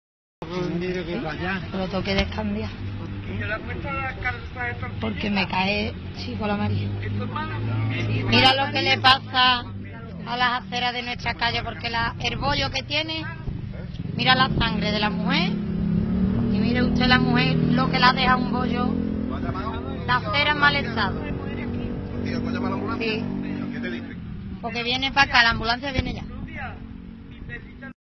Caída de una mujer en plena calle (Jerez)
Vídeo: Foto Denuncia Jerez.